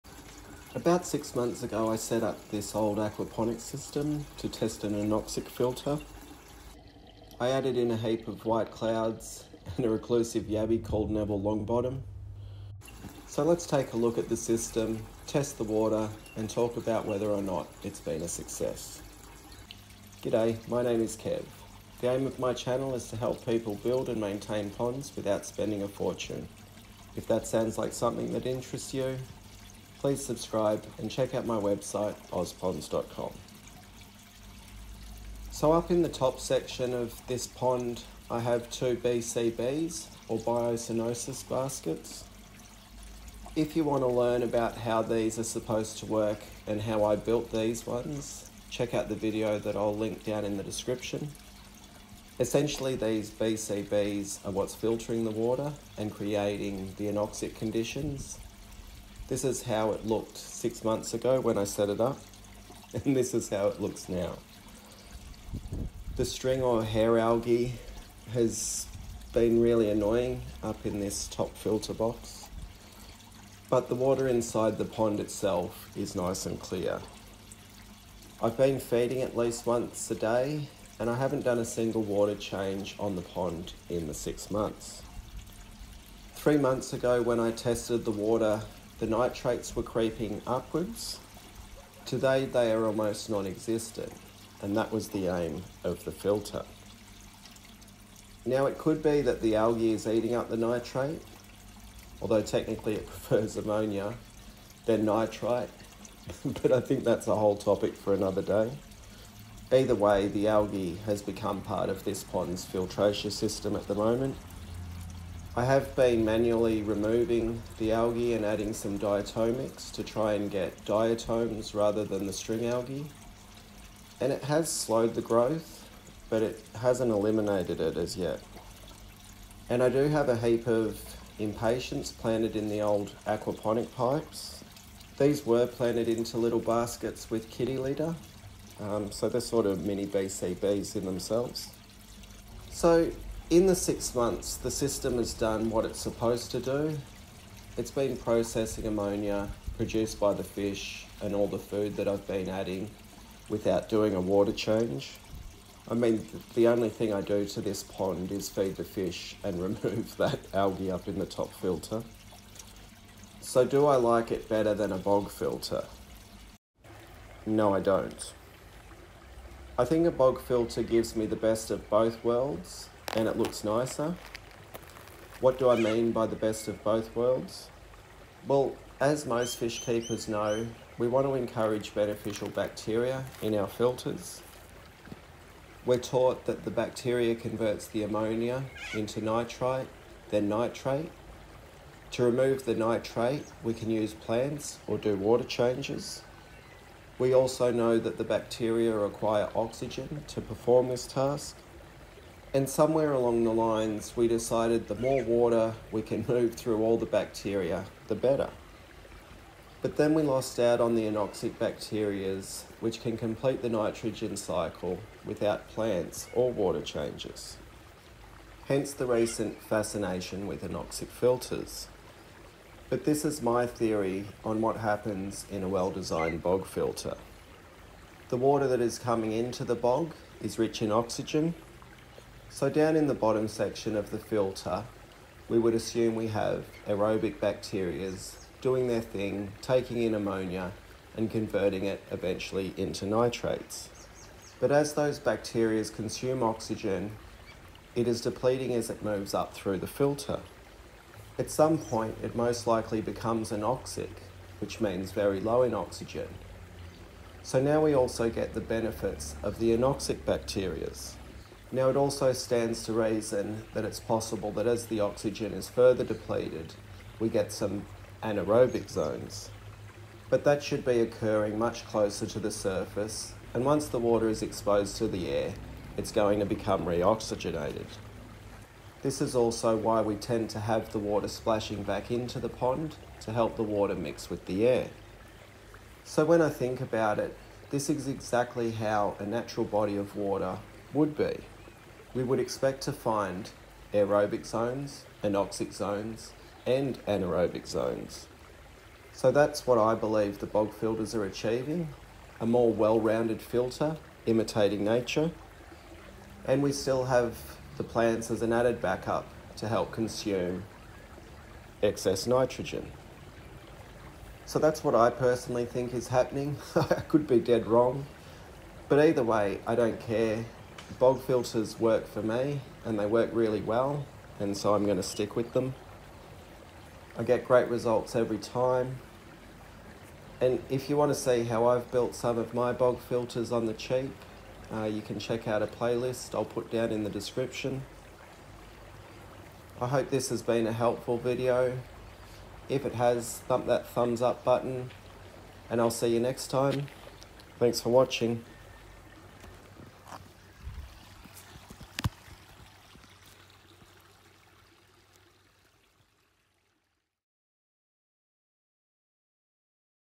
Anoxic pond. 6 month update
A quick video showing what's happening with the anoxic filter pond after 6 months. If you want to see how this was built and learn a little about the science, here’s the video